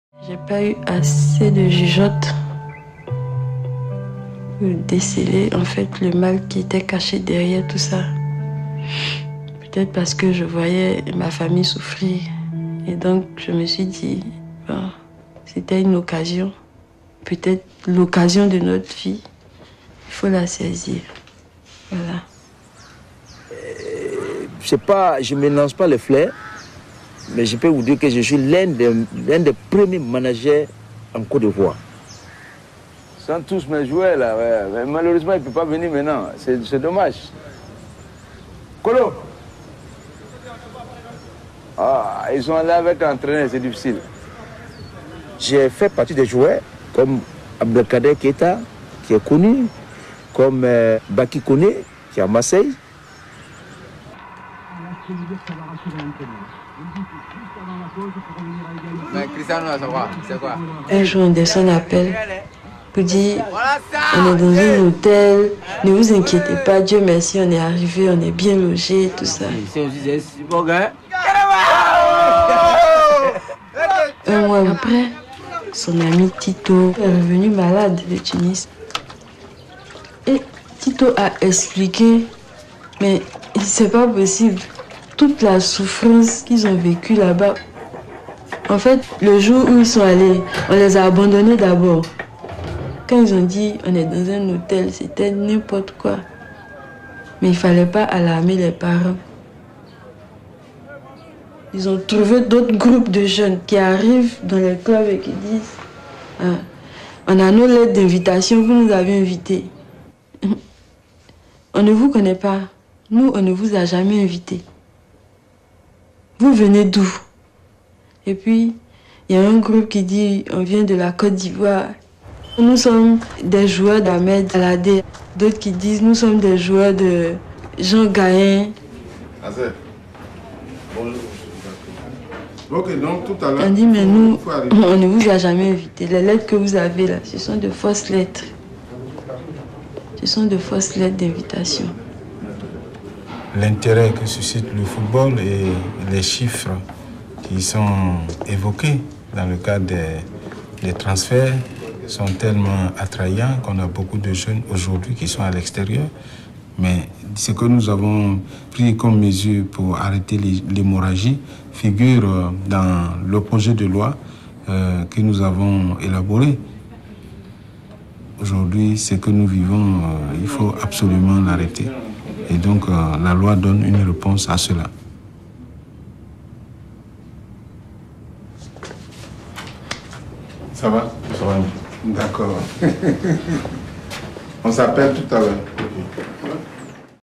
Human Trafficking African Football Players Fake Talent Organisations mp4
Young African Football players lured to Europe to play by fake agents